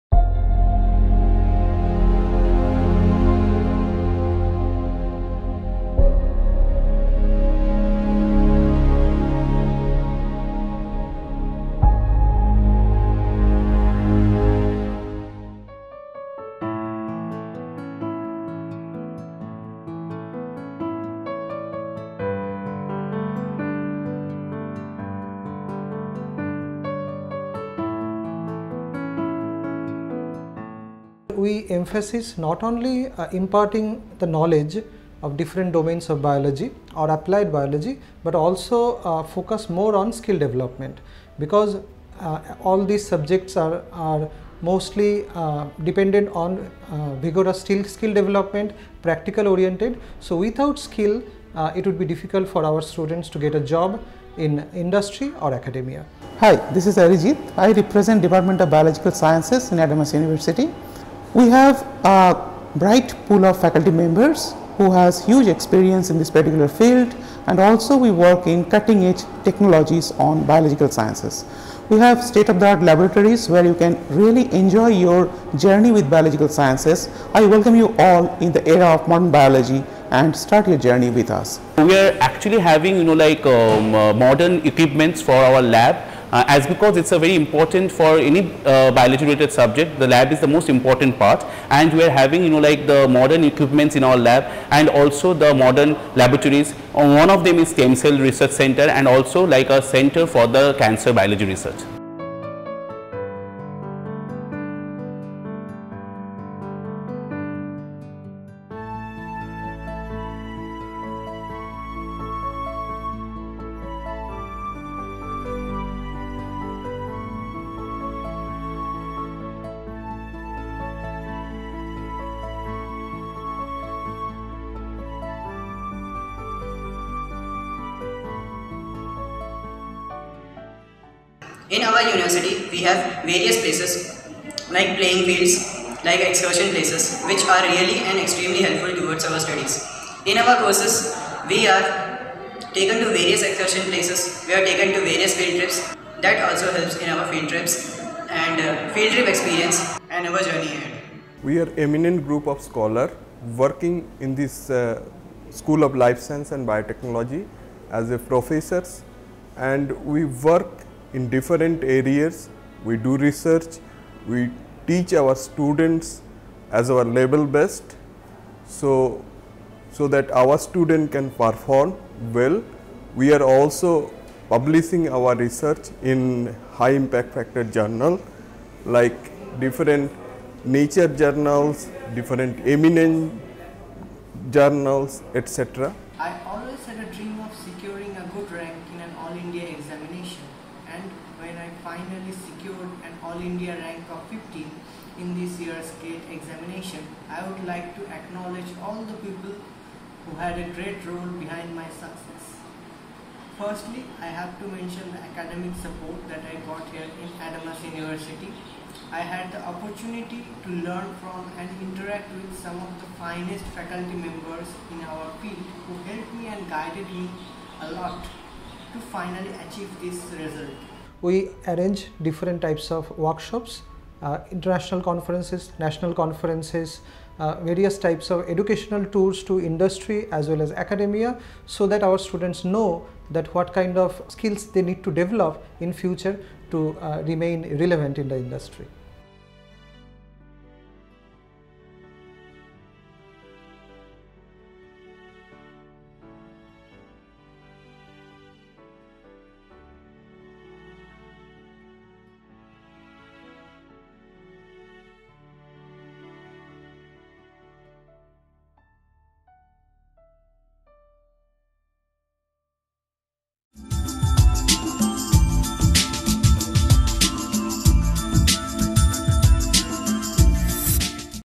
School of Life Science & Biotechnology | Adamas University
Advancement in technology is helping the bio science sector to grow daily which is leading to more and more avenues and jobs. Bio science & biotechnology is a booming sector and will continue to rise in the near future. The School of Life Science & Biotechnology at AdamasUniversity is home to unique B.Tech and B.Sc. programs that are breaking standards in the bio science & biotechnology sector. Watch this video to know more and for admissions